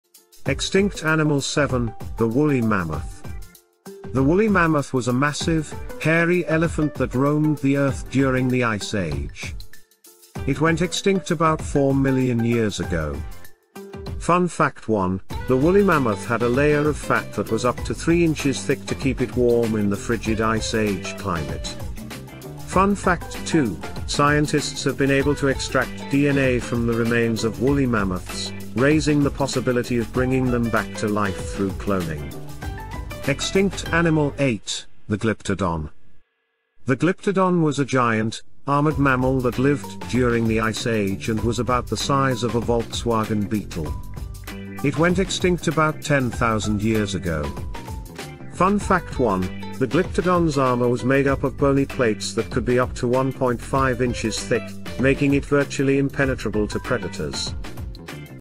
Extinct Animals Fascinating and Frightening Creatures You're Glad Are Gone creature extinct Part 4
Extinct Animals Fascinating and Frightening Creatures You're Glad Are Gone
Looking for a thrilling and educational journey through the animal kingdom's dark past? Look no further than our latest video! Join us as we explore 10 fascinating and frightening creatures that once roamed the Earth but are now thankfully extinct. From the massive and fearsome Sabre-Toothed Tiger to the armoured Glyptodon, we'll take you on a wild ride through prehistory to discover the fascinating facts and unique features of these creatures. So buckle up and get ready to learn about some of the most intriguing animals that once ruled the world!